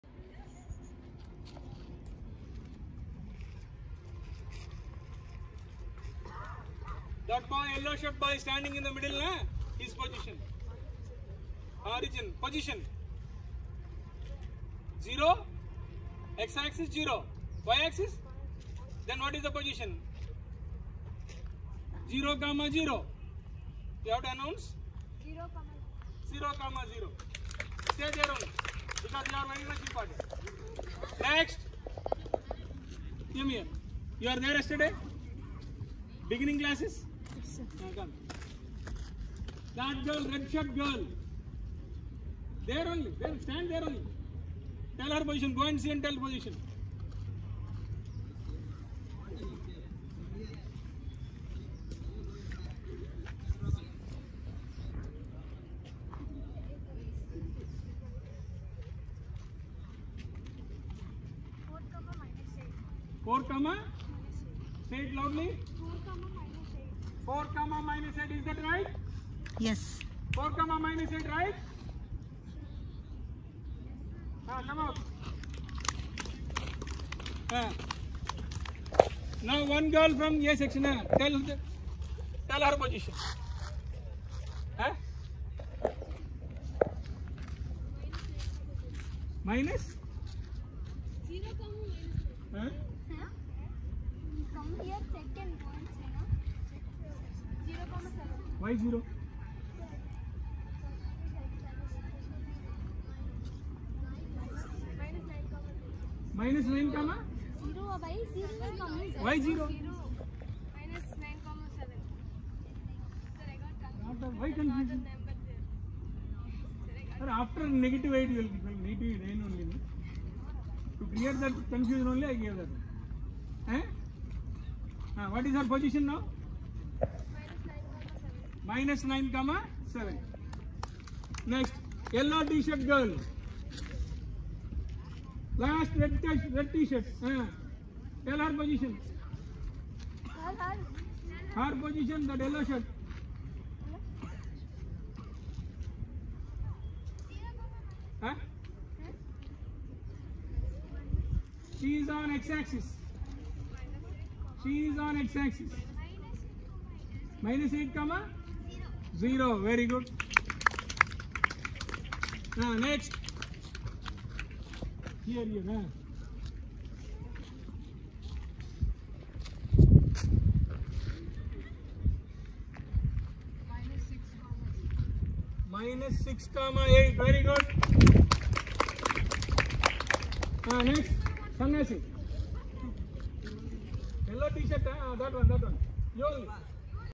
fun
Fun with mathematics
A game to understand plotting and finding the position of points on flat surfaces using coordinate geometry